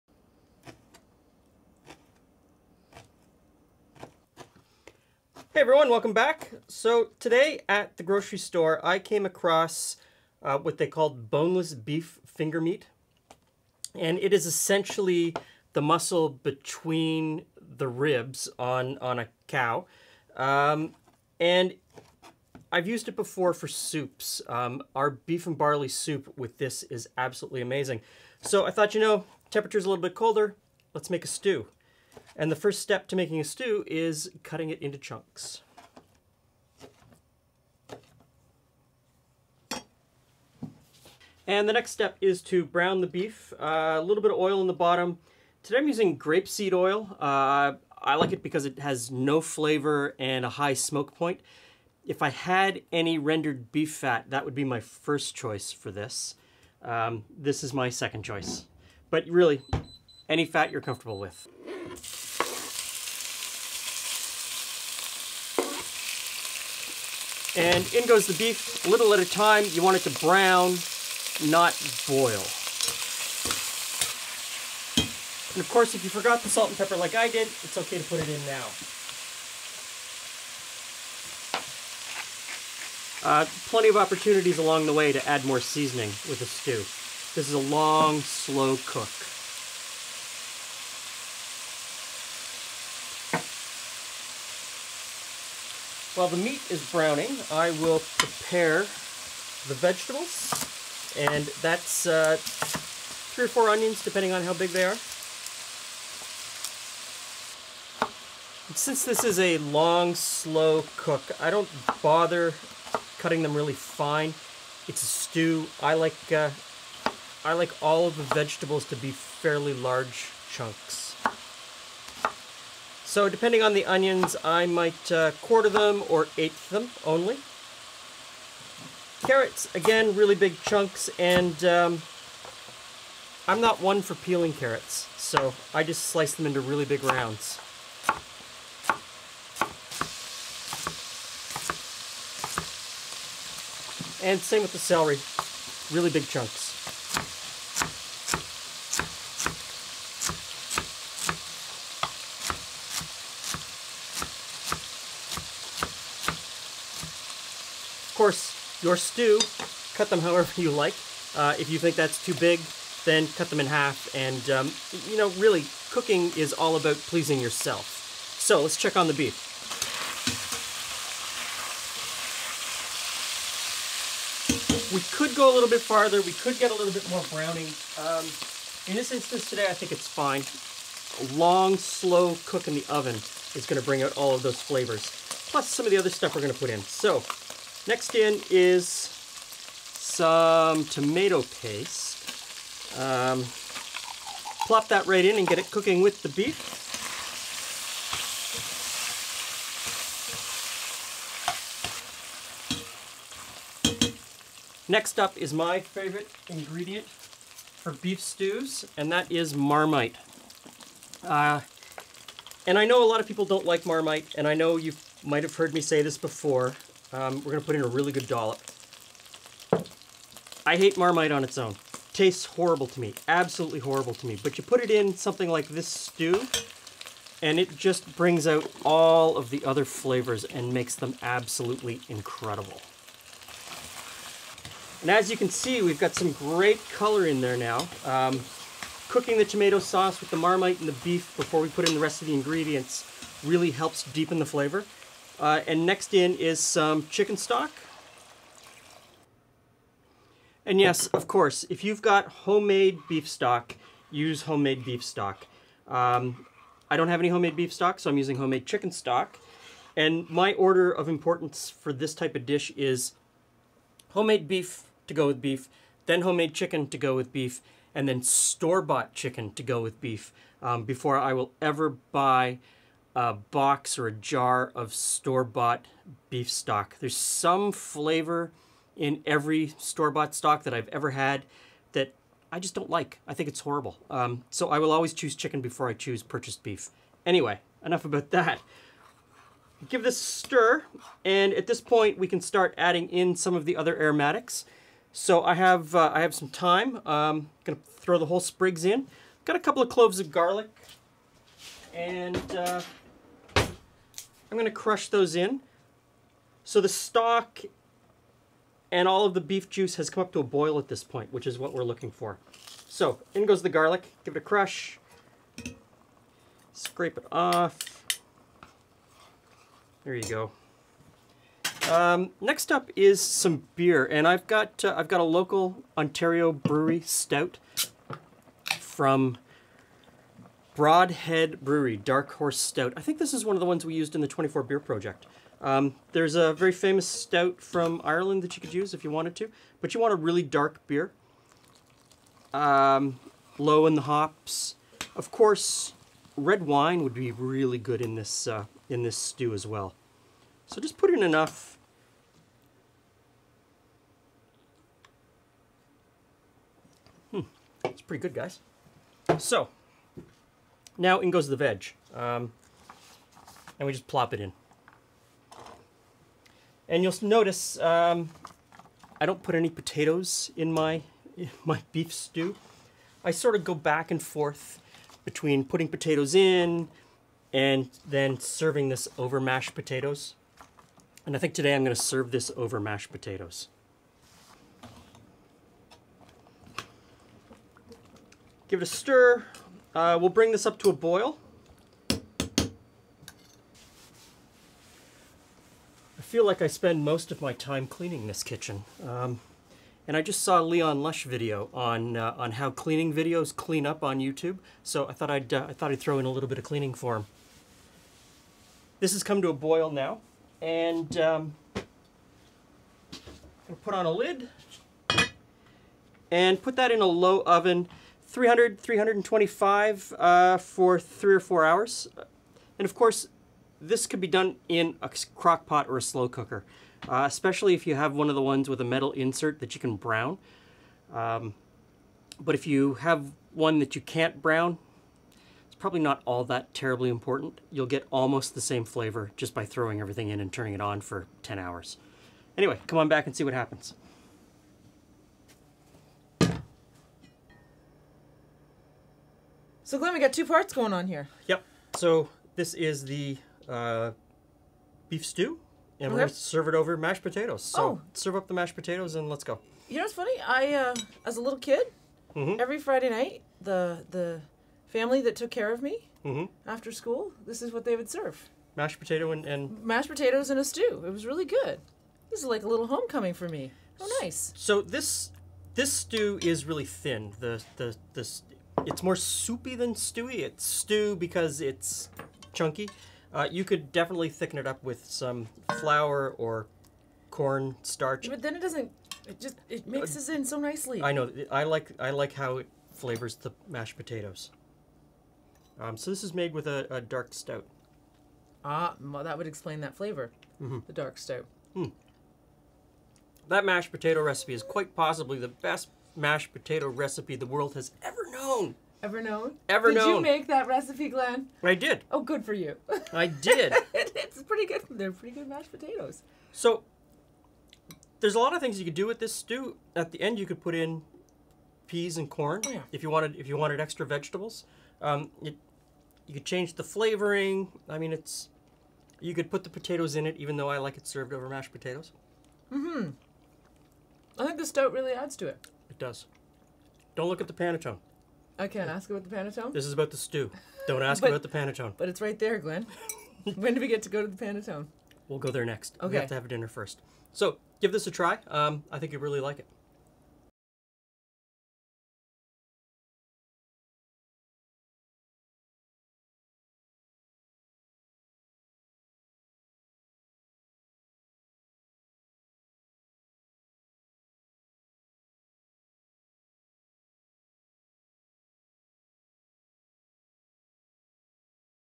🔵 Winter's Coming Beef And Stout Stew - Beef Stew
Our Winter's Coming Beef And Stout Stew Recipe is a perfect beef stew for those colder winter nights. This hearty beef stew is great served over mashed potatoes

Ingredients:
1 Kg (2 pounds) stewing beef
2 - 3 Tbsp oil
Salt and pepper to taste
60 mL (1/4 cup) tomato paste
15 - 30 mL (1 -2 Tbsp) Marmite or Vegemite
500 mL (2 cups) chicken or beef stock
2-4 cloves garlic, minced
4 sprigs of fresh thyme
1 can stout, or dark beer
2 -4  onions, chopped
2 big carrots, chopped
2 -3  ribs celery, chopped

Method:
Preheat oven to 325ºF
Cut up beef into 1" cubes.
Season beef with salt and pepper.
Heat a heavy cast iron Dutch oven on high, and coat bottom with oil.
Brown the beef in batches.
Place all of the beef back in the Dutch oven, and still on high add the tomato paste.
Stir in the tomato paste and cook in the oil and beef juices for a minute or two before adding the Marmite.
Continue frying the beef, tomato, and Marmite until you've built up a really great fond.
Deglaze the pan with the chicken stock.
Add the garlic, thyme, beer, onion, carrot, and celery.
Bring to a gentle boil, cover and place in the oven for 3-4 hours.

Le Gourmet TV Is Now - Glen & Friends Cooking!